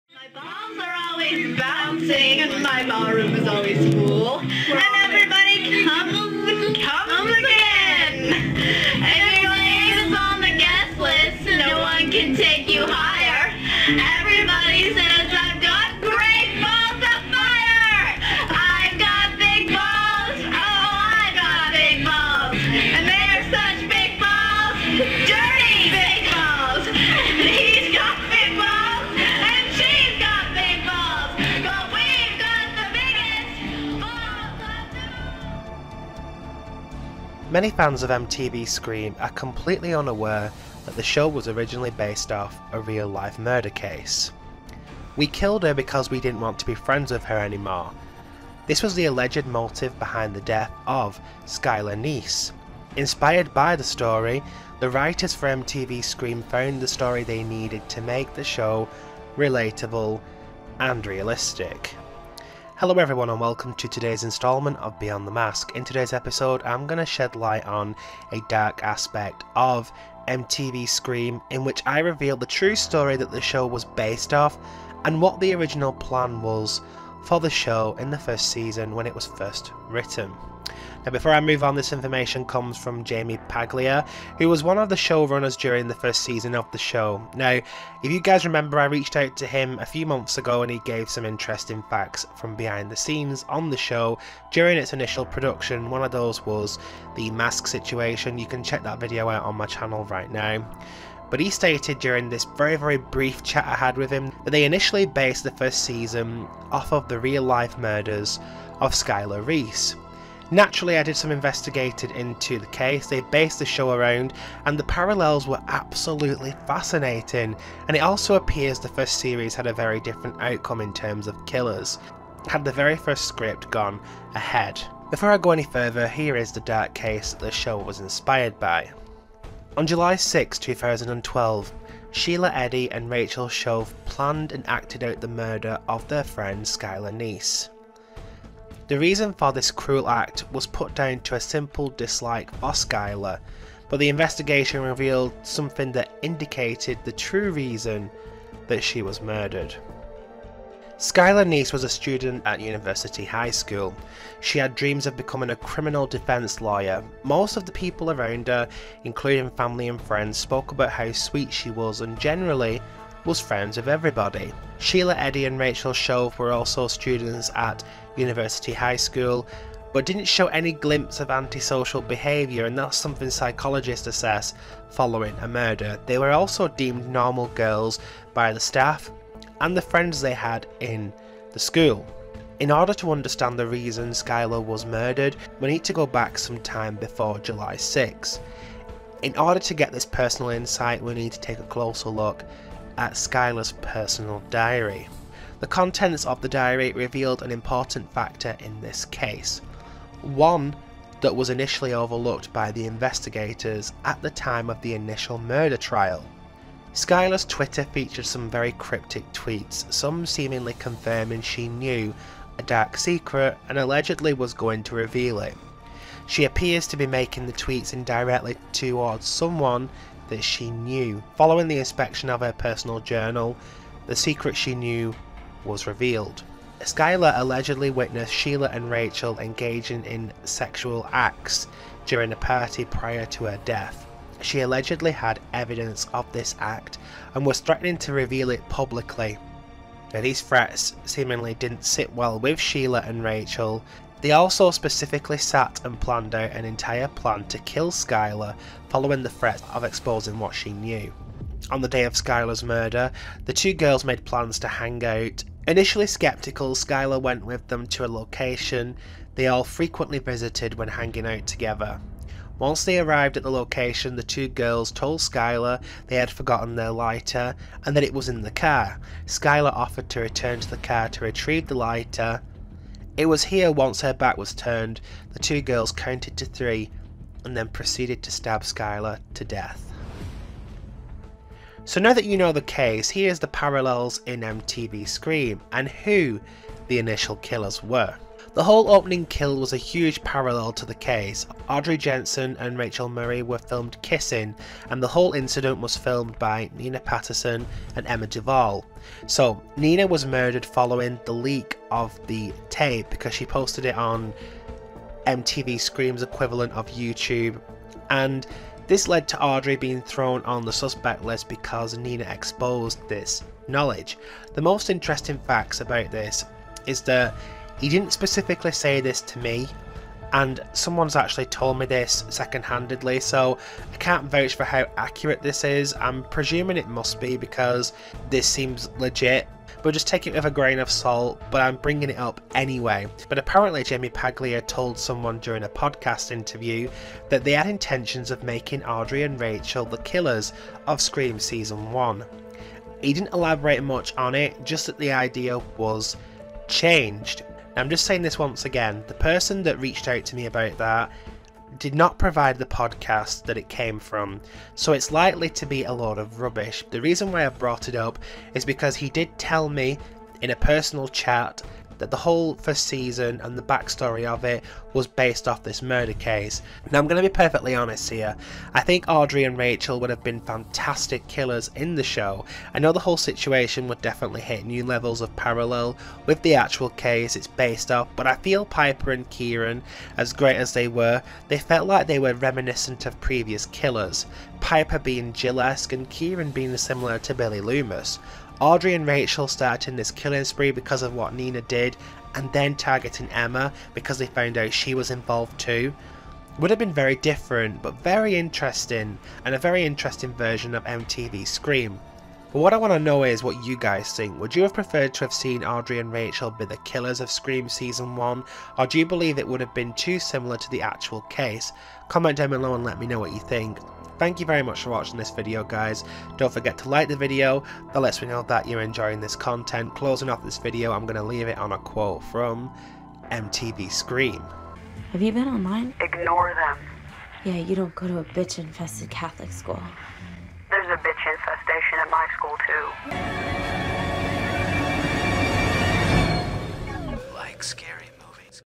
The CANCELLED MTV's SCREAM Killers & The Real Murderers they were based on...
MTV's Scream may have provided us with two great killers, but initially the idea was much different. A large amount of the first season were actually based on real events. The murder of Skylar Neese served as one of the main writing-points for the show & really allowed show-runners Jaime Paglia & Jill Blotevogel to push the boundaries between reality and fiction. In today's video, I reveal the alleged details about the TV Show killers & discuss in detail how the storyline drew parallels to the original events in 2012.